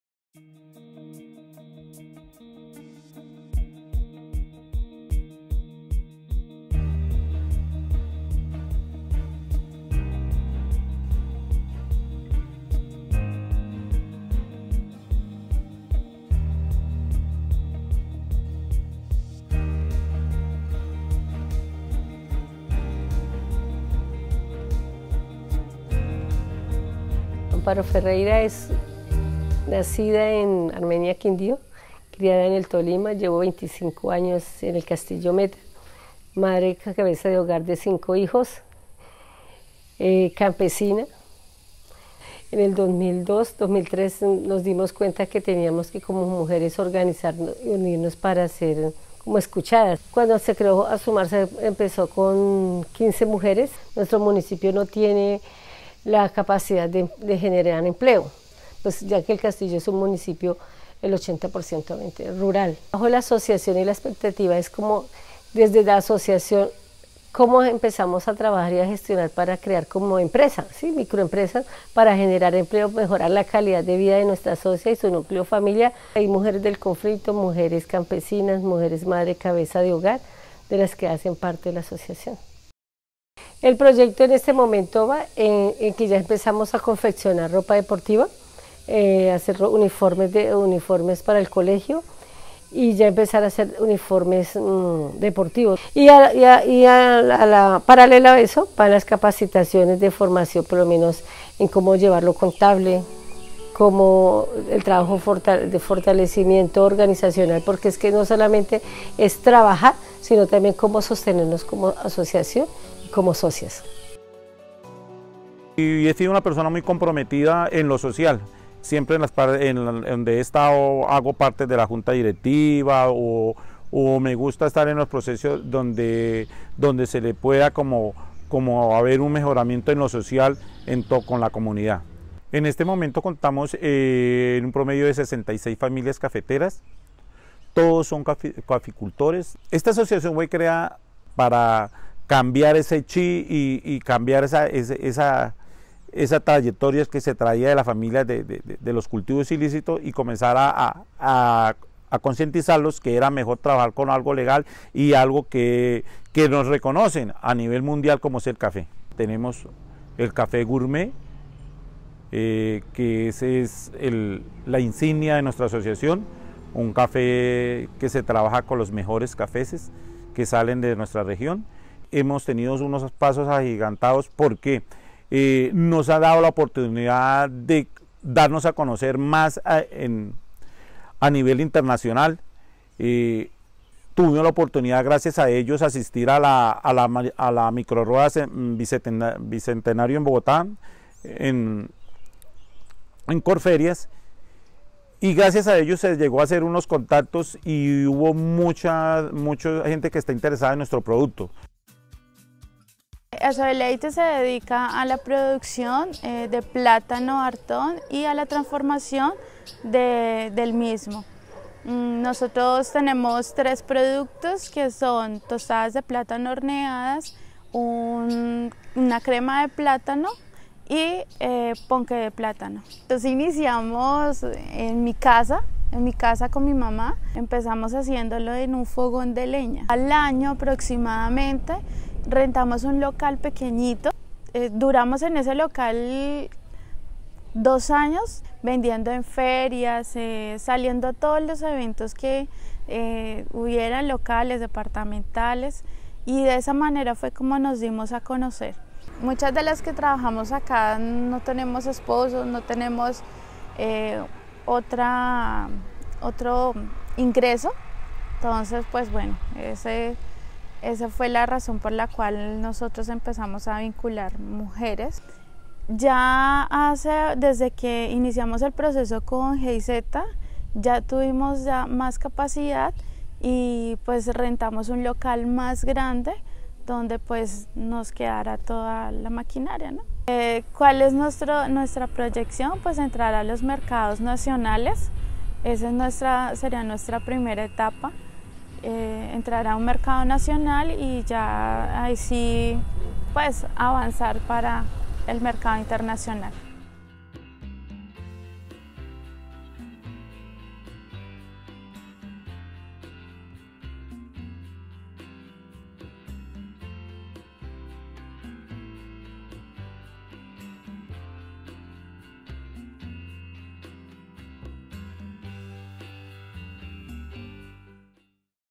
Creando para avanzar
Asociaciones de Productores en el Meta que aumentan los ingresos de sus negocios